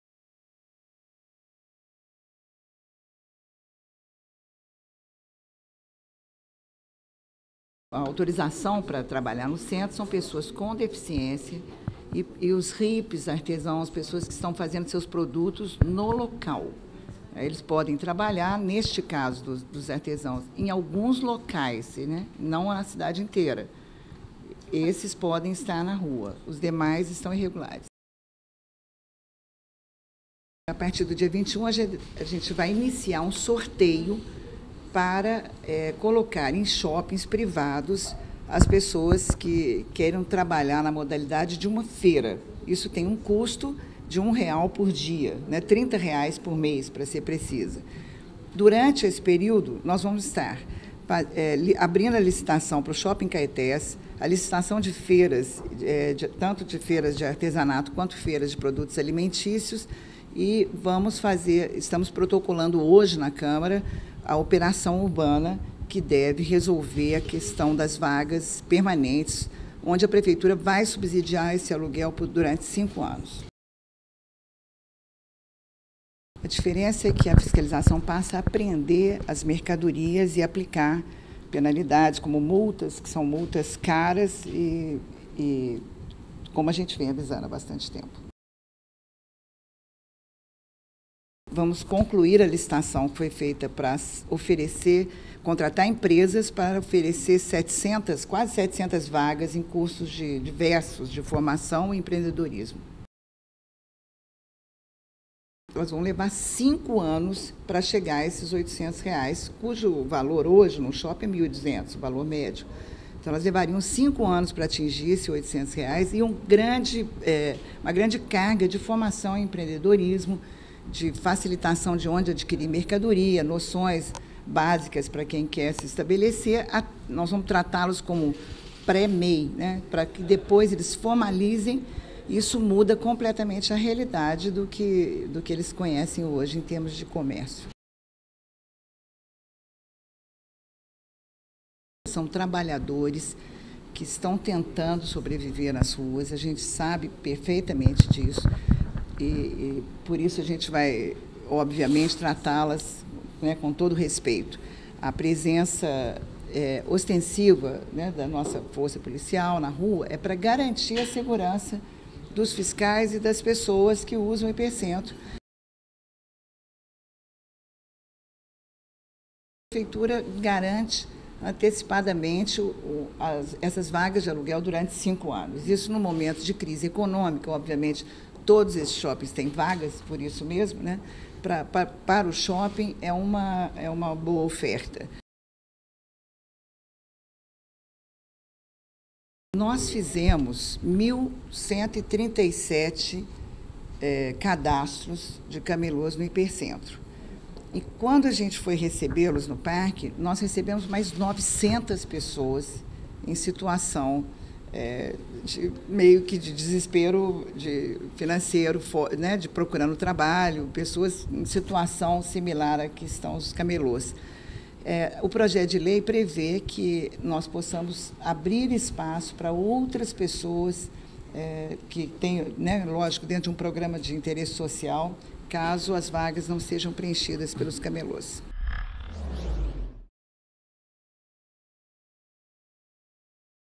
Coletiva sobre Plano de Revitalização do Hipercentro de BH
A secretária municipal de Serviços Urbanos, Maria Caldas, concedeu, nesta segunda-feira, dia 3,  no COP- BH, entrevista coletiva sobre o Plano de Revitalização do Hipercentro de BH. Confira no vídeo as informações fornecidas.